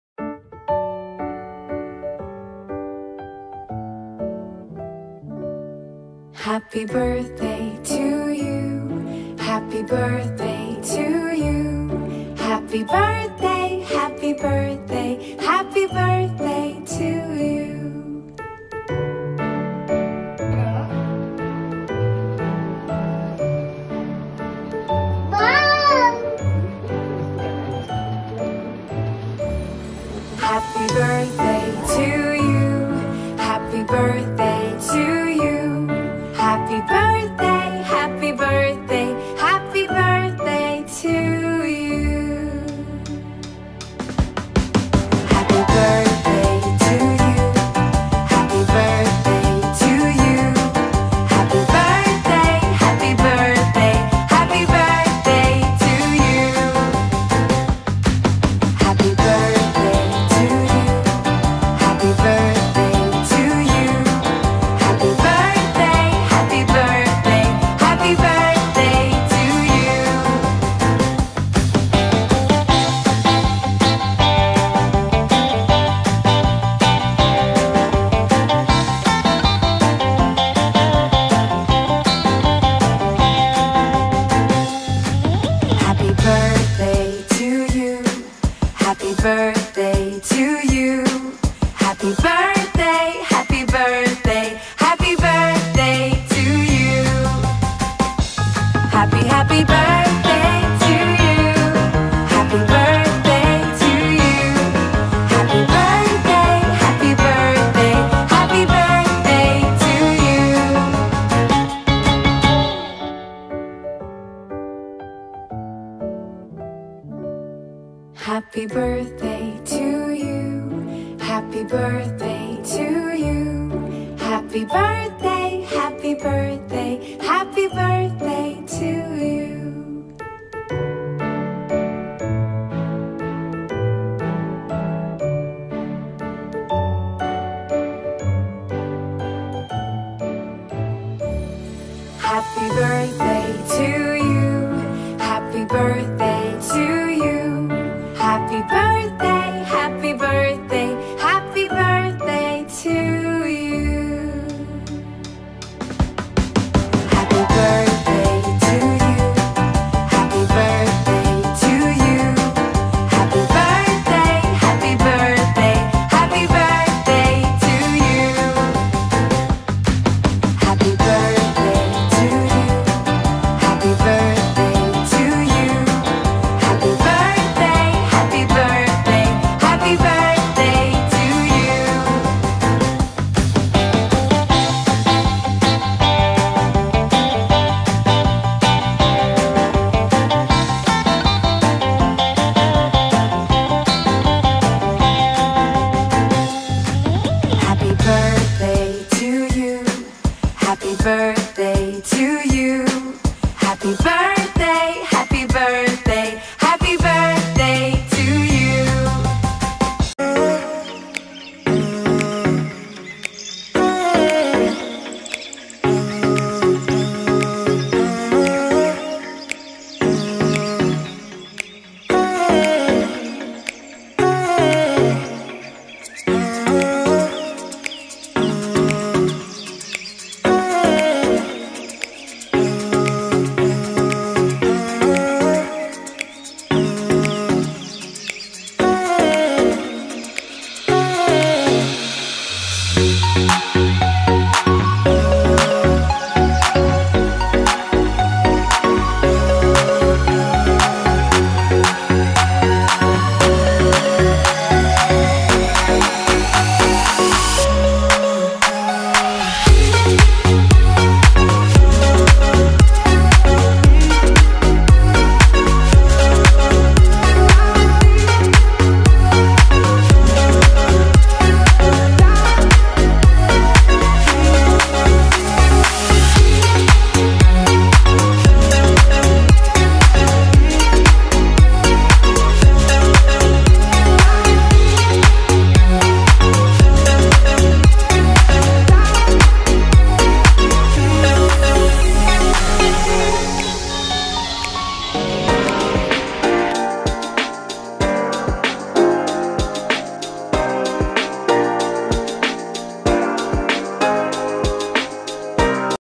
Lishanth Birthday Party || Enjoyed a lot || Meghan shows
Hello everyone,
we enjoyed a lot in my brother birthday 🎂 we are so happy on that day i gave him 3 gifts because bis age is 3 so I decided to give 3 if u want to see what are the gifts go and check it in video he is so happy at his birthday celebrations 🍾.

Editor
Meghan sai